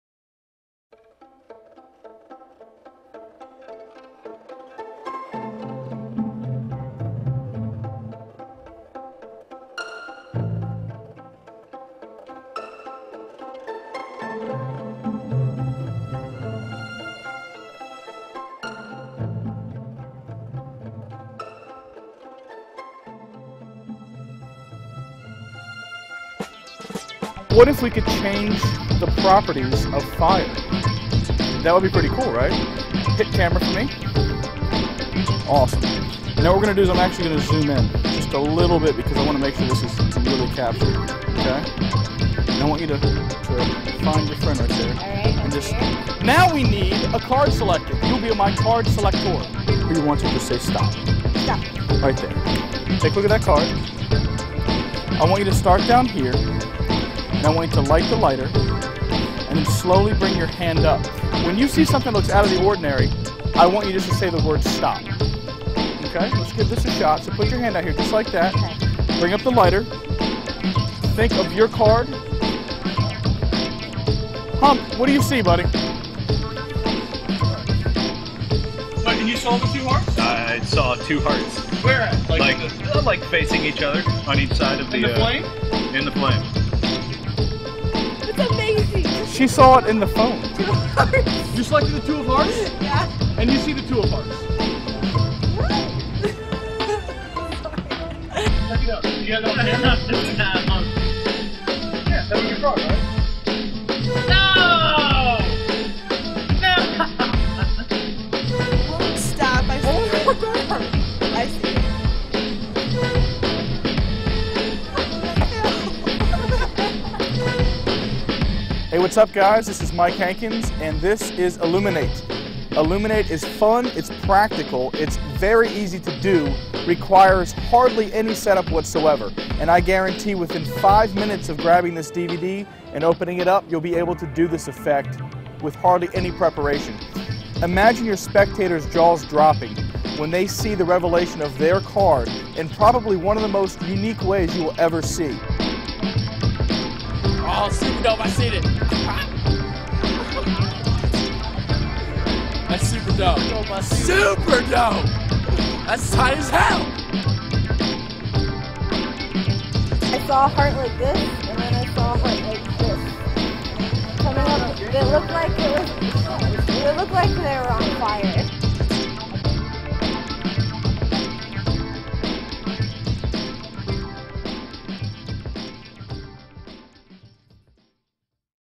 Illumin8 w/ Gimmick & DVD - M. Hankins
A card is selected and remembered. A lighter is borrowed. Introduce your phone, your digital camera, your FLIP HD, or your camcorder. Have the spectator document what is about to happen.
So what happens? Imagine being able to light a borrowed lighter, and cause the FLAME itself to form the shape o... For More Details and Purchase,